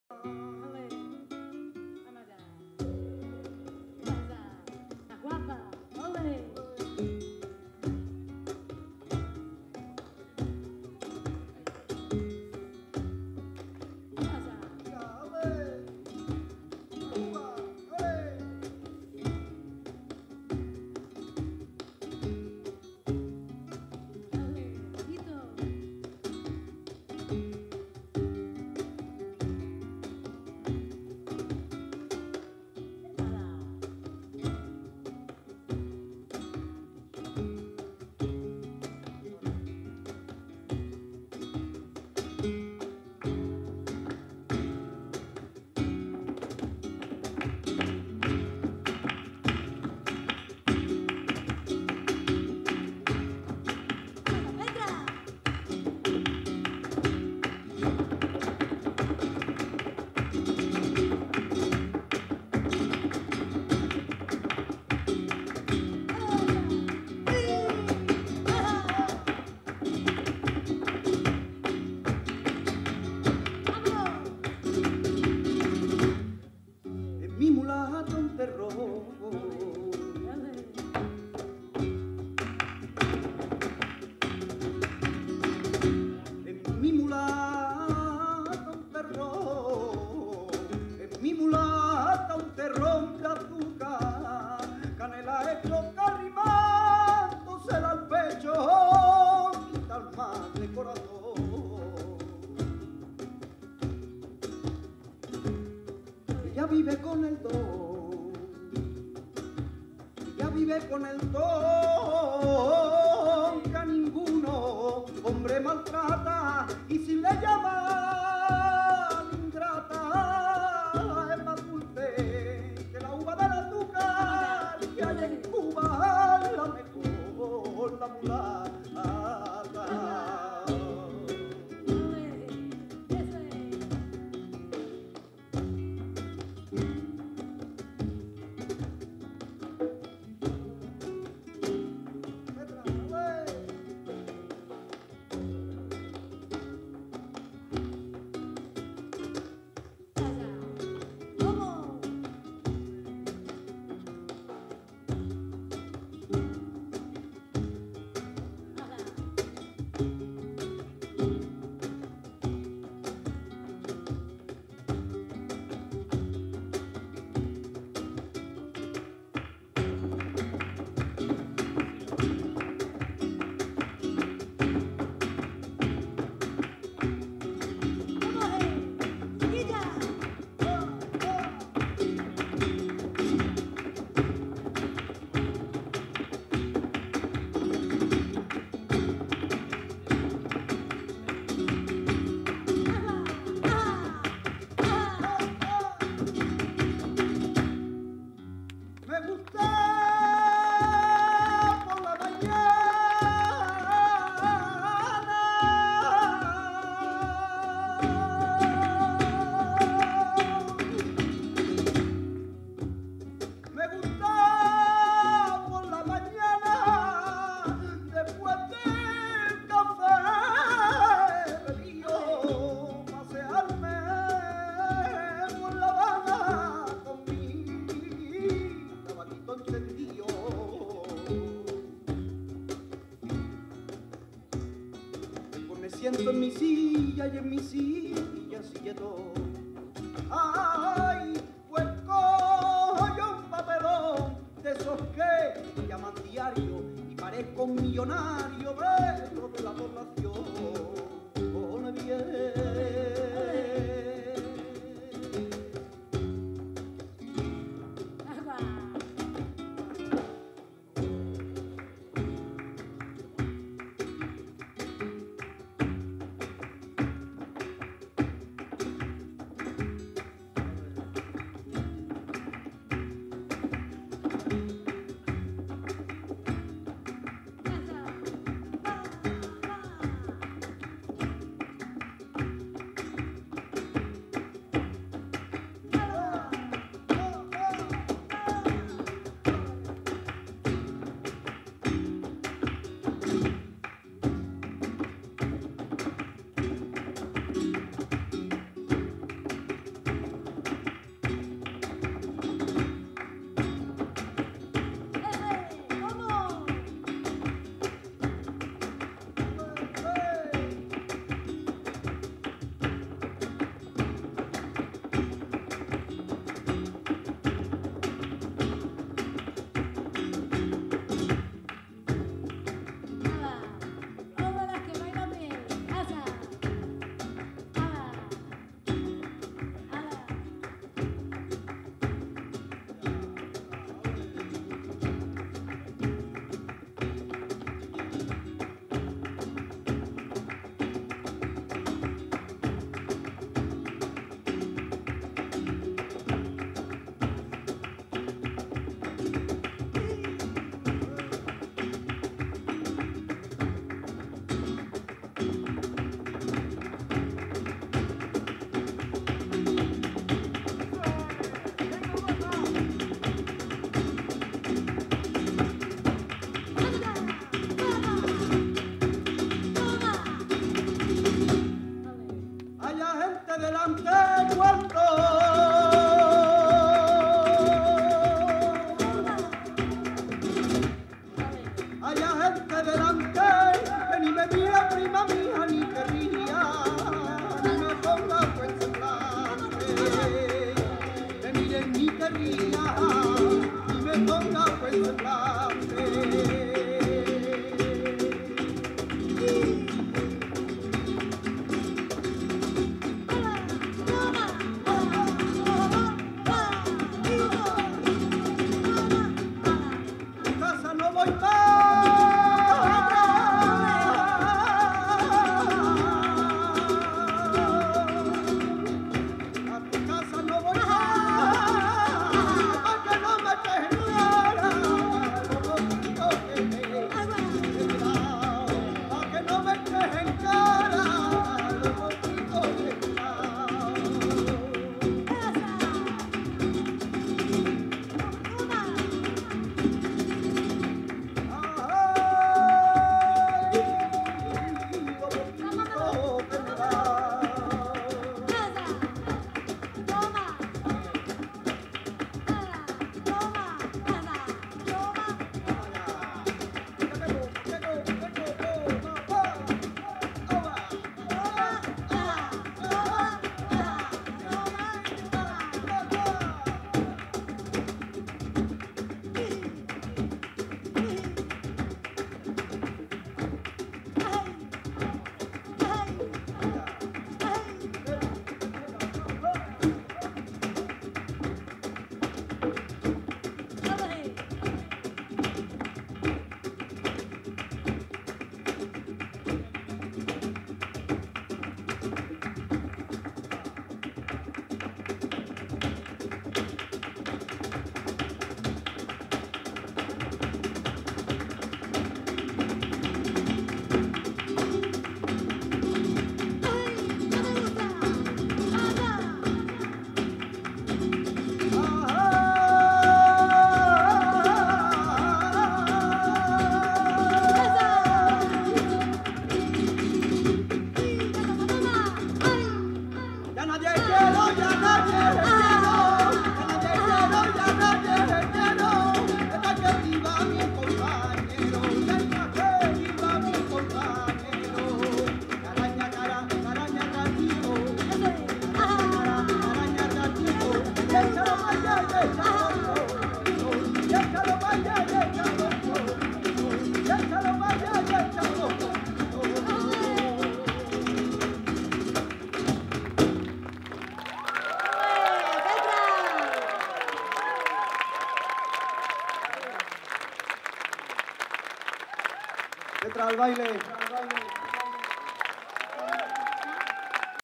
Babel Sound Festival
Guajira

Cante: Pere Martinez
Guitar: Ruben Flores
Percussion: Xoan Sanchez
Palmas: Violeta Barrio
Dance: Petra Borocz